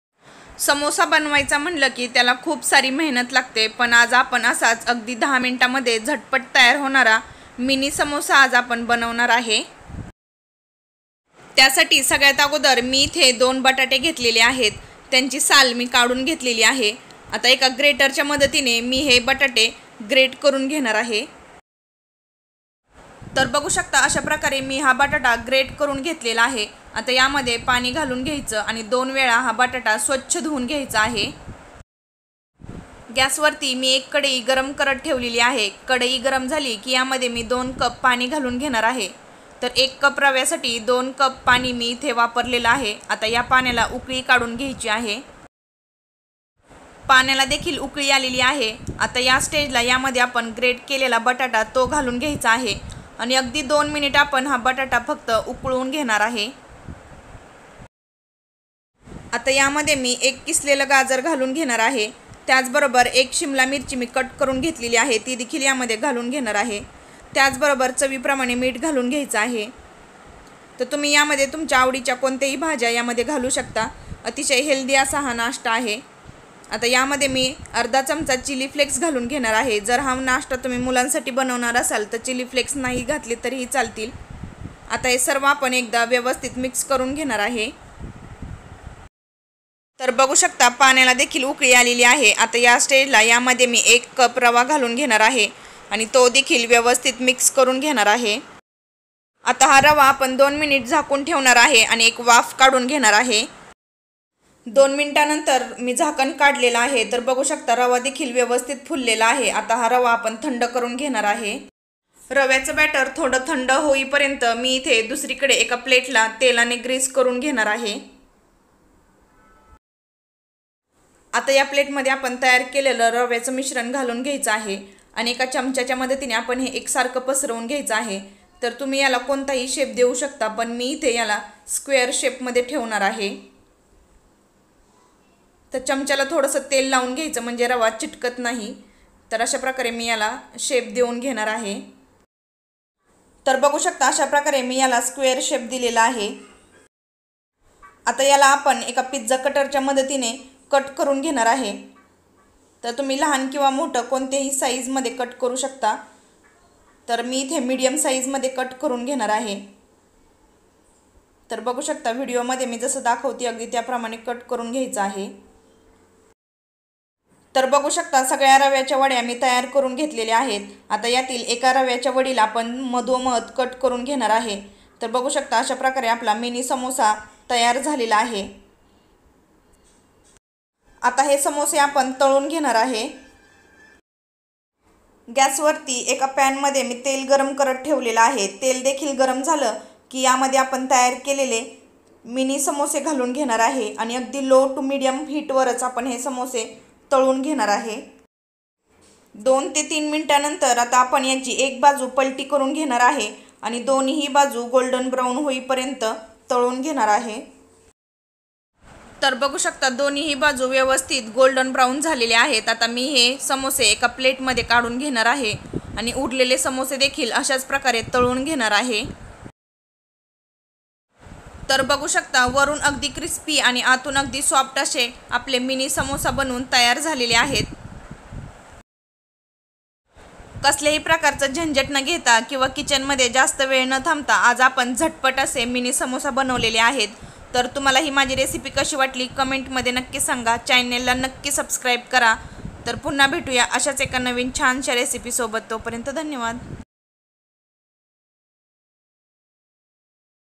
1 वाटी रवा वापरुण मुलांना आवडेल असे रवा समोसा/ Breakfast Recipe
न रवा, न बटाटे वापरता झटपट तयार होणारे मिनी समोसा/Instant Brekfast